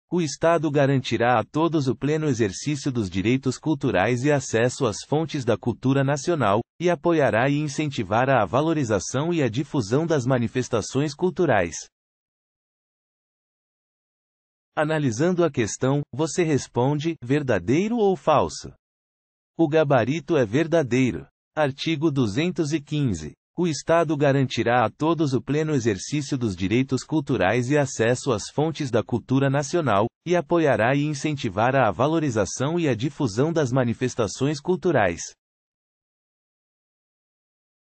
QScon Direito: [Art. 215, CF] IDECAN 2019 - Assistente em Administração (IF-AM)
Art. 215, CF | IDECAN 2019 | Assistente em Administração | (IFAM)

O Estado garantirá a todos o pleno exercício dos direitos culturais e acesso às fontes da cultura nacional, e apoiará e incentivará a valorização e a difusão das manifestações culturais.

Gabarito: Verdadeiro